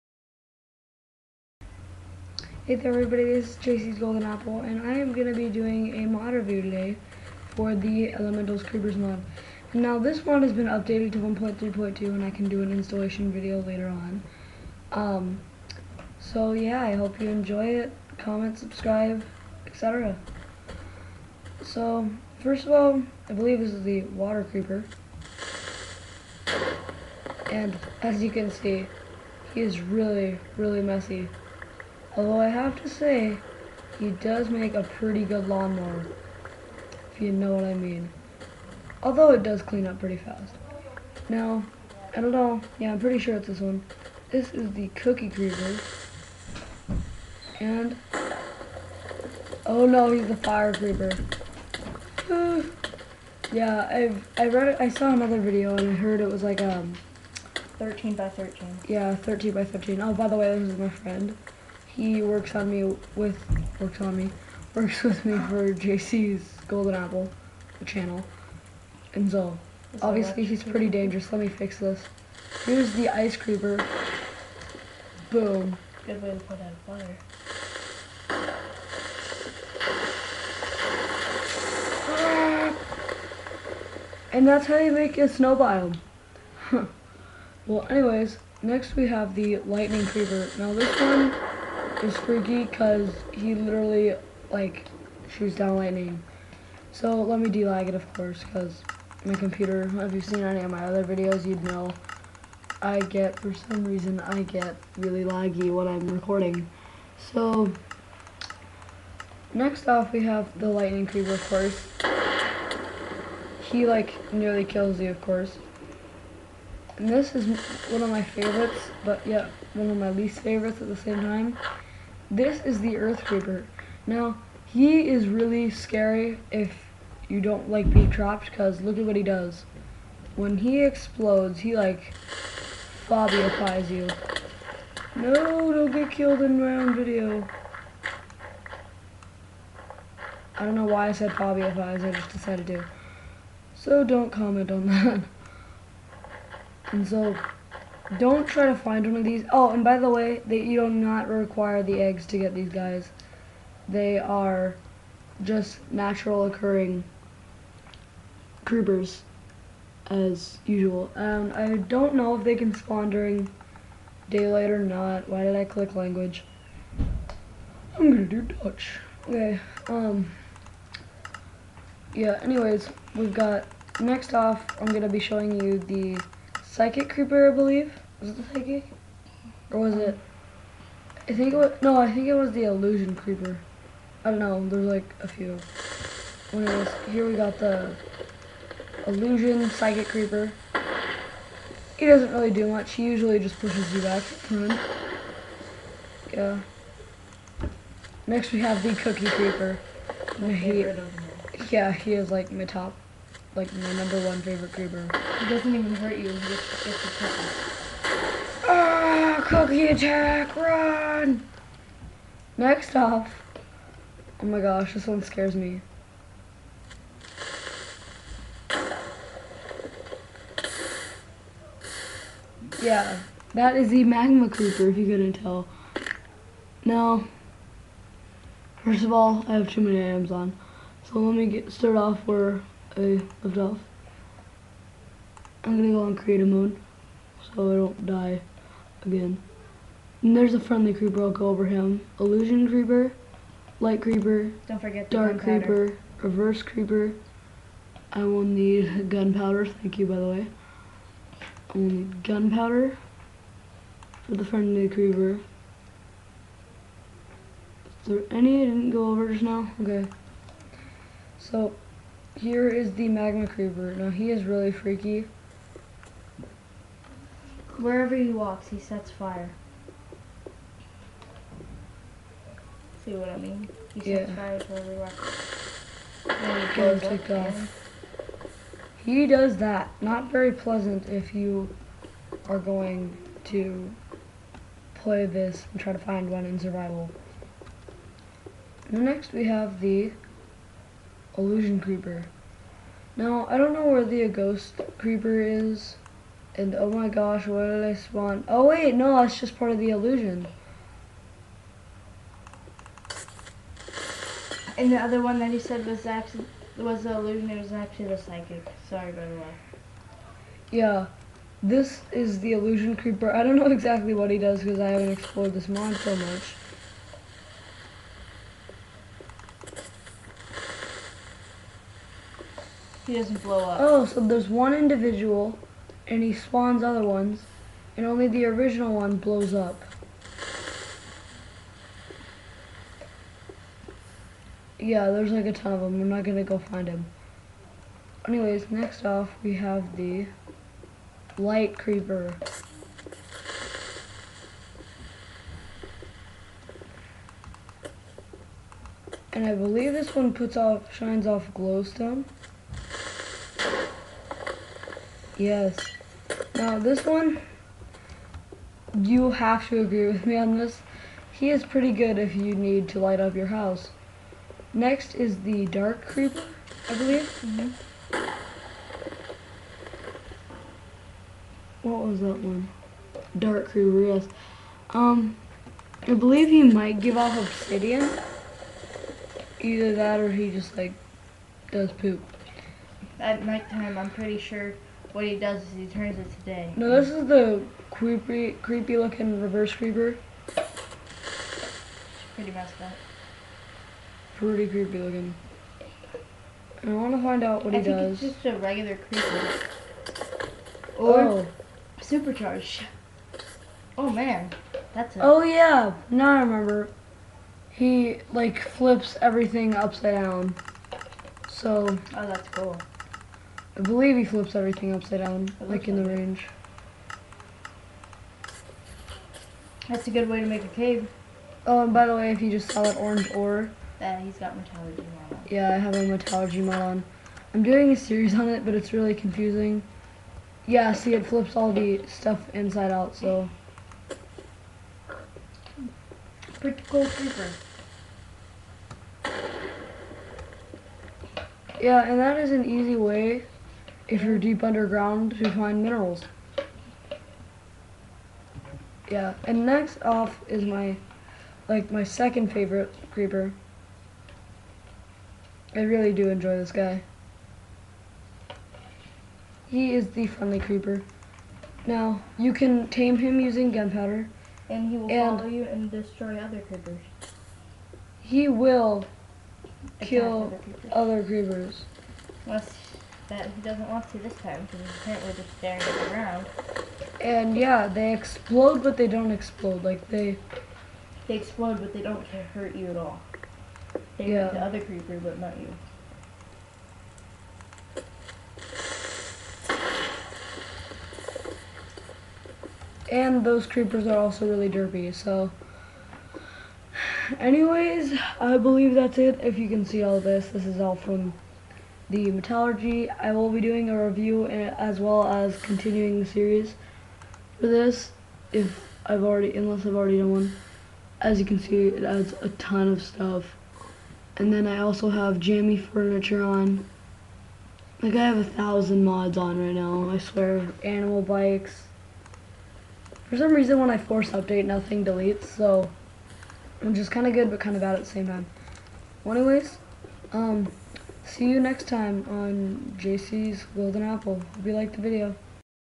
Elemental Creepers mod review
This is a pretty awesome mod reccomended you get it ill do a installation vid if i get comments